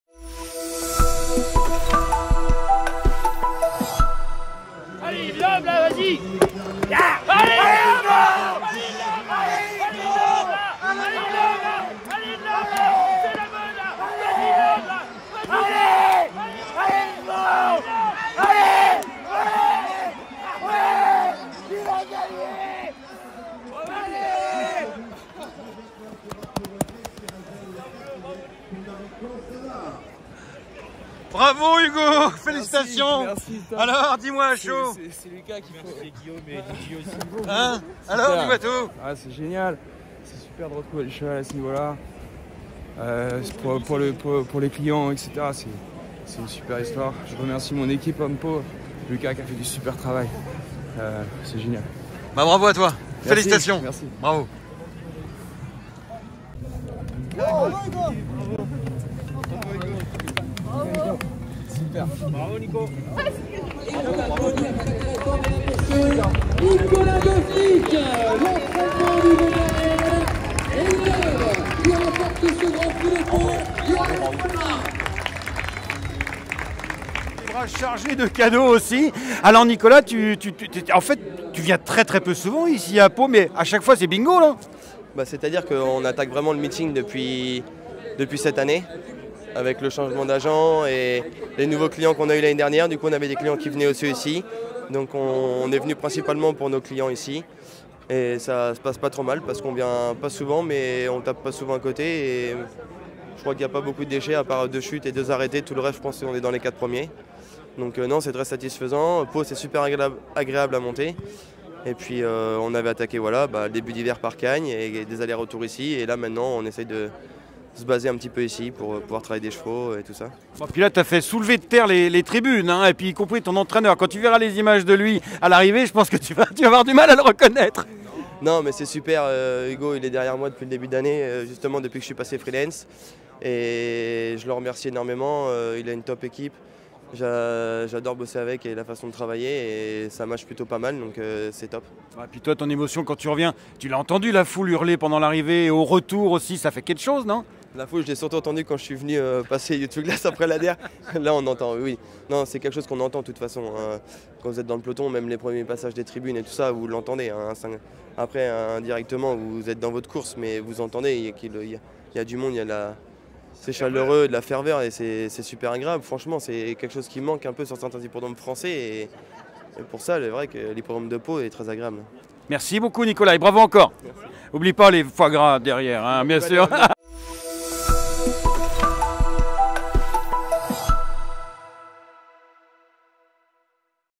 Grand Prix de Pau 2024 : Hugo Merienne et Nicolas Gauffenic sur le toit des Pyrénées avec In Love
Sous les yeux de 14 394 personnes venues se délecter d'une grande et belle journée de sport au pied des Pyrénées, In Love et Nicolas Gauffenic ont réussi à décrocher la palme dans le 137ème Grand Prix de Pau (Gr.3), l'un des principaux temps forts du meeting d'hiver béarnais. De quoi donner le "smile" à tout l'entourage de cet AQPS par Great Pretender, à commencer par son jeune metteur au point, Hugo Merienne.